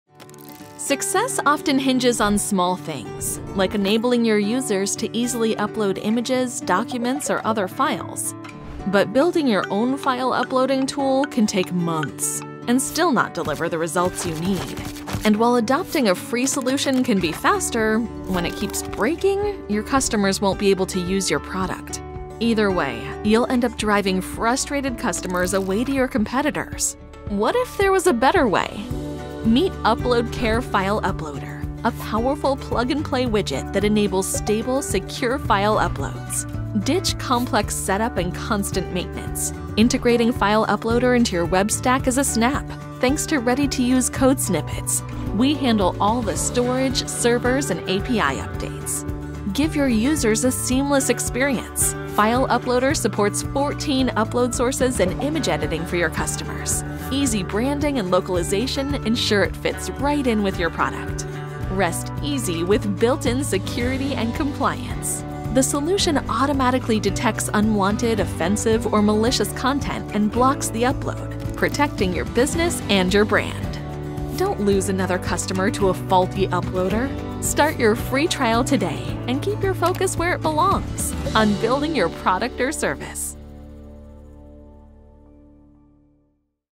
Uploadcare File Uploader
File Uploader is a plug-and-play widget and comprehensive API for scalable and secure file uploads. Get the file upload functionality you need through quick integration and configuration, instead of months of development.

Success often hinges on small things, like enabling your users to easily upload images, documents, or other files. But building your own file uploading tool can take months and still not deliver the results you need. And while adopting a free solution can be faster, when it keeps breaking, your customers won’t be able to use your product. Either way, you’ll end up driving frustrated customers away to your competitors.

What if there was a better way? Meet Uploadcare File Uploader — a powerful plug-and-play widget that enables stable, secure file uploads.

— Ditch complex setup and constant maintenance. Integrating File Uploader into your web stack is a snap, thanks to ready-to-use code snippets. We handle all the storage, servers, and API updates.

— Give your users a seamless experience. File Uploader supports 14 upload sources and image editing for your customers. Easy branding and localization ensure it fits right in with your product.

— Rest easy with built-in security and compliance. The solution automatically detects unwanted, offensive or malicious content and blocks the upload, protecting your business and your brand.

Don’t lose another customer to a faulty uploader.